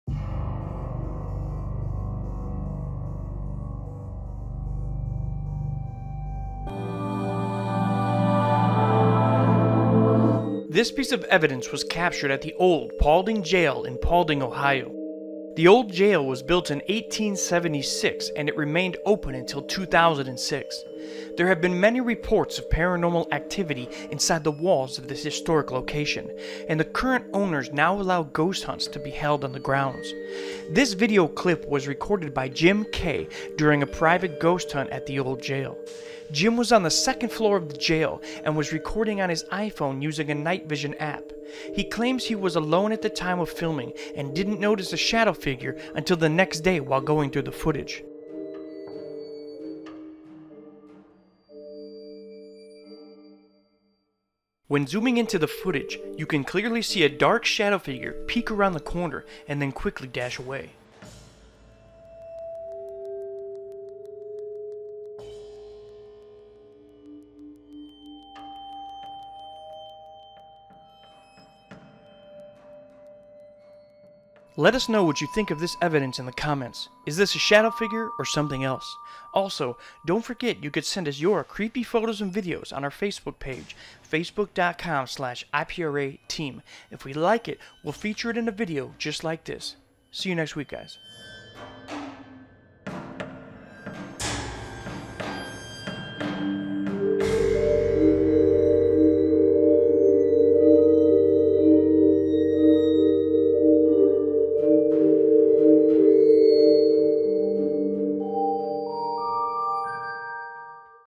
Real Ghost Caught On Video Camera Inside Haunted Jail
A man catches a shadow figure ghost inside of the Old Paulding Jail in Ohio

Sign up for our newsletter and get an email as soon as we have a new upload